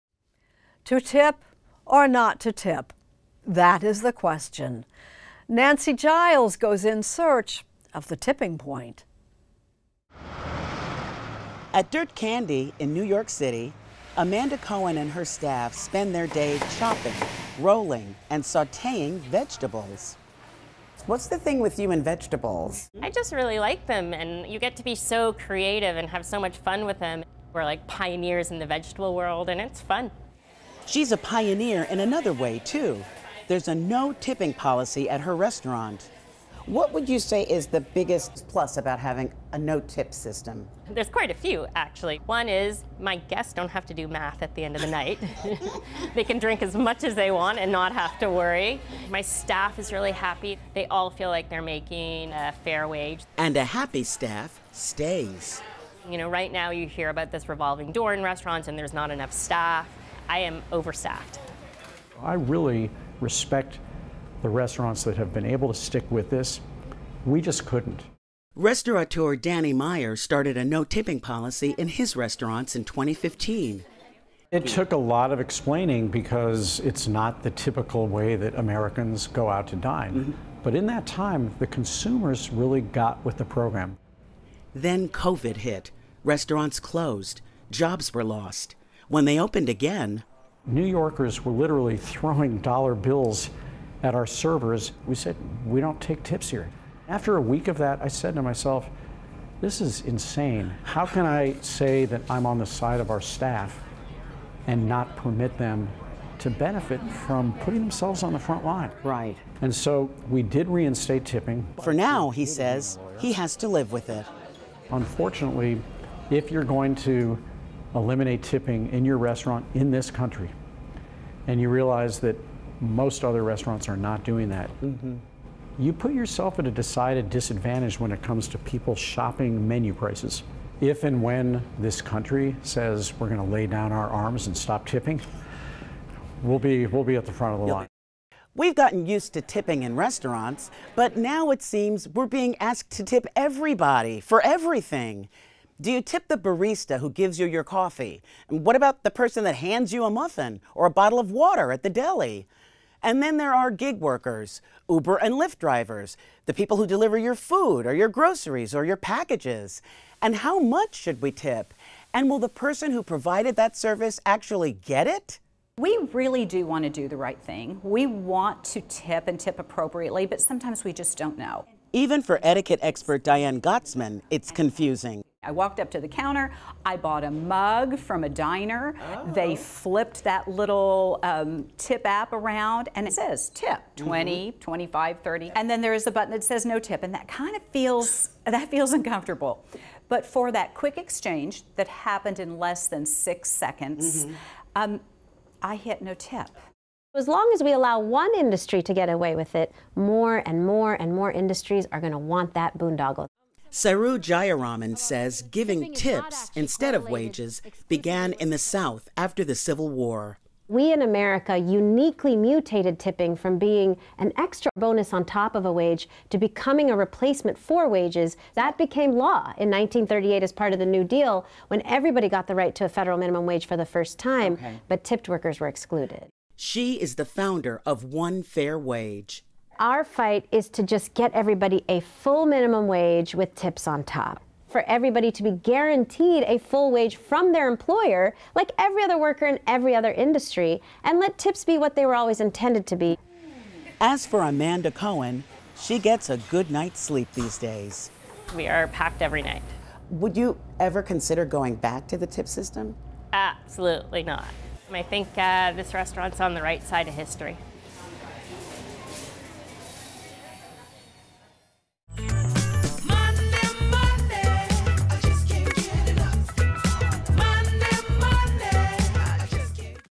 Tips on tipping
Americans have gotten used to tipping in restaurants, but these days it seems customers are being asked to tip for everything, everywhere. (And in the case of some restaurants, not at all!) Correspondent Nancy Giles tries to get some answers on the protocols of tipping from restaurateurs Danny Meyer and Amanda Cohen, and etiquette expert Diane Gottsman.